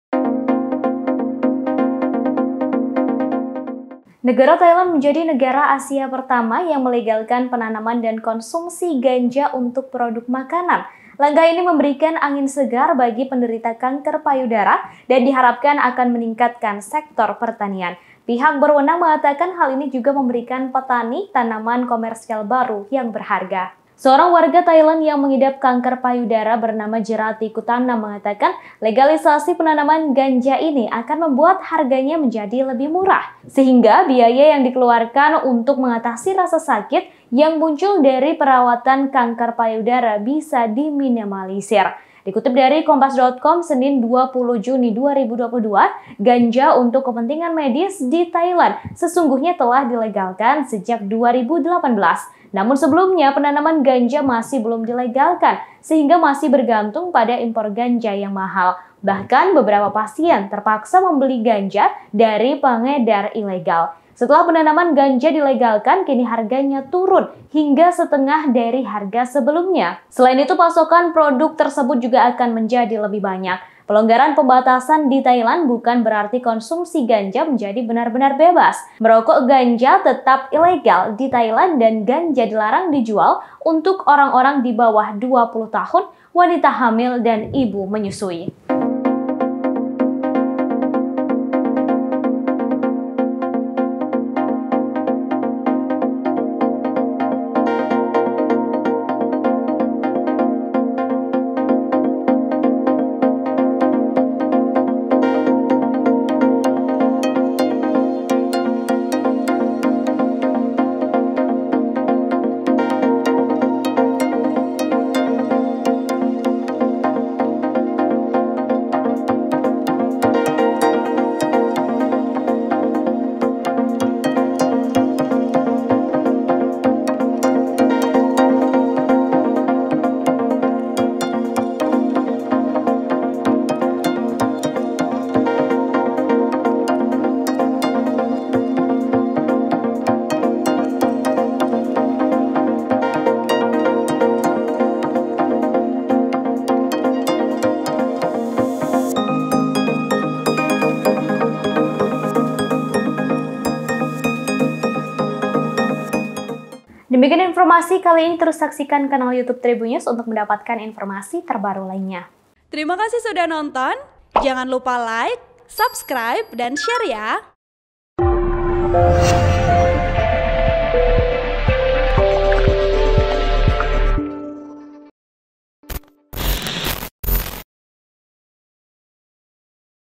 Penanaman Ganja Legal di Thailand, Angin Segar untuk Penderita Kanker dan Sektor Pertanian
TRIBUN-VIDEO.COM - Negara Thailand menjadi negara Asia pertama yang melegalkan penanaman dan konsumsi ganja untuk produk makanan.

Langkah ini memberikan angin segar bagi penderita kanker payudara dan diharapkan akan meningkatkan sektor pertanian.

Pihak berwenang mengatakan, hal ini juga memberikan petani tanaman komersial baru yang berharga.

Seorang warga Thailand yang mengidap kanker payudara bernama Jiratti Kuttanam mengatakan, legalisasi penanaman ganja ini akan membuat harganya menjadi lebih murah.

Sehingga biaya yang dikeluarkan untuk mengatasi rasa sakit yang muncul dari perawatan kanker payudara bisa diminimalisir.

Dikutip dari Kompas.com, Senin (20/6/20220, ganja untuk kepentingan medis di Thailand sesungguhnya telah dilegalkan sejak 2018.

Namun sebelumnya, penanaman ganja masih belum dilegalkan sehingga masih bergantung pada impor ganja yang mahal.

Bahkan, beberapa pasien terpaksa membeli ganja dari pengedar ilegal.

Setelah penanaman ganja dilegalkan, kini harganya turun hingga setengah dari harga sebelumnya.

“Saya telah mengonsumsi ganja secara teratur sehingga saya tidak merasa sakit,” katanya.

Selain itu, pasokan produk tersebut juga akan menjadi lebih banyak.

Pelonggaran pembatasan di Thailand bukan berarti konsumsi ganja menjadi benar-benar bebas.

Merokok ganja tetap ilegal di Thailand dan ganja dilarang dijual untuk orang-orang di bawah 20 tahun, wanita hamil, dan ibu menyusui.

(Tribun-Video.com/Kompas.com)

Artikel ini telah tayang di Kompas.com dengan judul "Thailand Legalkan Tanam Ganja, Penderita Kanker Payudara Dapat Angin Segar", Klik untuk baca
Penulis : Danur Lambang Pristiandaru
Editor : Danur Lambang Pristiandaru
Host: Ratu Sejati
Video Production: Adam Sukmana